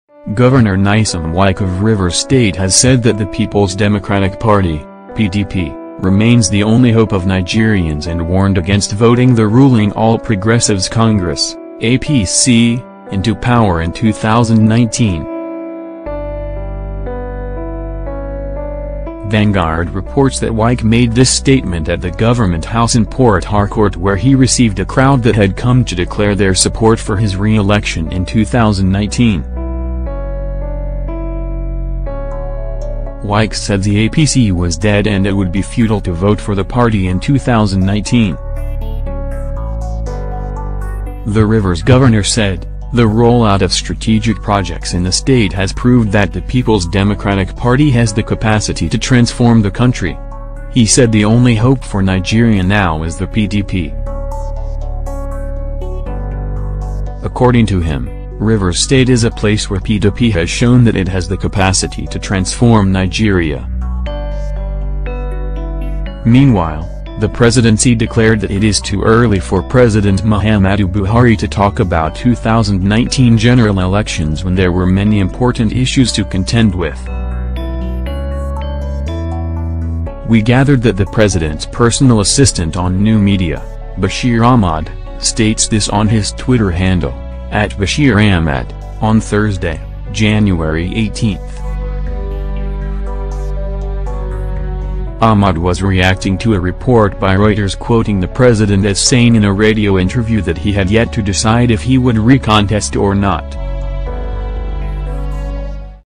PDP is the only hope for Nigerians - Wike
- Governor Wike has called on Nigerians to support the PDP in 2019

- He said the opposition party remains the only hope for Nigerians

- The Rivers governor claimed the APC has failed the people